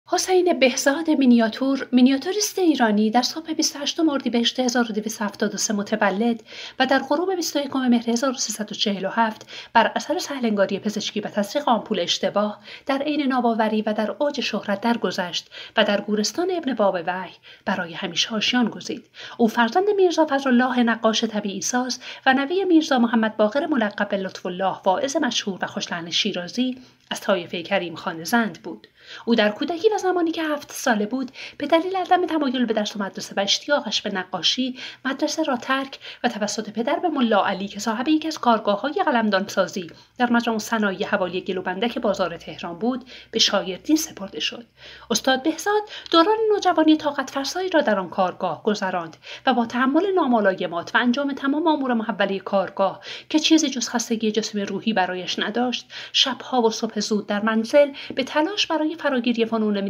حسین بهزاد یکی از تاثیرگذارترین هنرمندان عرصه نگارگری: افسونگری با موهای سپید
حسین بهزاد یکی از تاثیرگذارترین هنرمندان عرصه نگارگری هنرمندی که ژان کوکتو نویسنده فرانسوی درباره وی چنین گفته است: «بهزاد پیغمبر افسونگری است از مشرق‌زمین داستان‌سرا. اگر همیشه مشرق‌زمین به قصه‌های هزار و یک شب و کاخ‌های طلایی و کنیزکان زیبایش برای ما داستان‌ها می‌گفته این بار، مردی با موهای سفید و چشمان بانفوذ و اندامی با خط‌ها و رنگ‌هایش اَشکال افسون‌کننده در مقابل چشمان ما گسترده ‌است. بدون شک در مینیاتور قرن ما، یک استاد از جهت قدرت طرح و رنگ‌آمیزی وجود دارد که او حسین بهزاد هنرمند ایرانی است».
حسین بهزاد فرزند میرزا فضل الله نقاش طبیعی ساز و نوه میرزا محمد باقر ملقب به لطف الله، واعظ مشهور و خوش لحن شیرازی از طایفه کریم خان زند بود.  پدرش در قلمدان‌سازی و نقاشی آبرنگ شهرتی داشت. وی تحصیلات خود را در مدرسه شرف ظفر آغاز اما پس از مدتی به دلیل علاقه به نقاشی، تحصیل را رها کرد. پدرش او را برای فراگیری مینیاتور به مجمع‌الصنایع نزد ملاعلی قلمدان‌ساز اصفهانی فرستاد. پس از درگذشت پدرش و ملاعلی قلمدان‌ساز اصفهانی، بهزاد از محضر حسین پیکرنگار بهره برد. در سال ۱۳۱۴ موسیو رابینو باستان‌شناس فرانسوی بهزاد را با خود به فرانسه برد و وی را به کار گرفت. در این هنگام تاجرهای کلیمی عتیقه‌فروش، دائم به‌طور محرمانه به وی مراجعه کرده و از او دعوت به کار می‌کردند زیرا می‌توانستند کارهای بهزاد را به عنوان کارهای قدیمی و عتیقه بفروشند.  اقامت وی در فرانسه، ۱۳ ماه طول کشید و در این مدت وی به موزه‌های لوور، گیمه و ورسای می‌رفت و آثار قدیمی مینیاتور ایرانی و شرقی را مطالعه می‌کرد. او این دوران را این‌طور توصیف می‌کرد: «من در این مسافرت شکفته شدم و خودم را شناختم و پنجه‌های من از آن پس شور و احساس نوظهور تازه‌ای یافت». در جای دیگری می‌گوید: «مطالعه من در سبک‌های مختلف نقاشی ایرانی و خارجی بدین منظور بود تا سبکی به ‌وجود بیاورم که ضمن داشتن خصایص ایرانی با هنر روز دنیا نیز منطبق باشد.» در ایران برای افراد مختلفی کار کرد. کمی بعد جنگ جهانی دوم شروع شد و متفقین که در تهران حضور پیدا کردند کارهای بهزاد را خوب می‌خریدند. گفته می‌شود وی نخستین نقاش مبتکر مینیاتور پس از صفویان بوده‌ است و این هنر را از گرایشی که در زمان قاجار به سمت نقاشی‌های روسی پیدا کرده بود به سمت نقاشی‌های ایرانی بازگرداند. بسیاری بر این باورند که هر چند او معتقد به پیروی از سبک خاصی در نگارگری نبود ولی ابداعات و تغییرات ایجادشده توسط او به‌وجود‌آورنده سبک خاص وی بود که در عین حال تاثیرگذار بر آثار نگارگران هم‌عصر خود بوده ‌است. مجلس شورای ملی در اردیبهشت ۱۳۳۷ تصویب کرد حقوق معینی به شکل مادام‌العمر از طرف دولت به حسین بهزاد پرداخت شود. "حسین بهزاد مینیاتور"، مینیاتوریست ایرانی در صبح بیست و هشتم اردیبهشت ماه سال هزار و دویست و هفتاد و سه خورشیدی متولد شد و در غروب بیست و یکم مهر ماه سال هزار و سیصد و چهل و هفت، بر اثر سهل انگاری پزشکی و تزریق آمپول اشتباه، در عین ناباوری و در اوج شهرت درگذشت و در گورستان ابن بابویه برای همیشه آشیان گزید.
یادش گرامی‌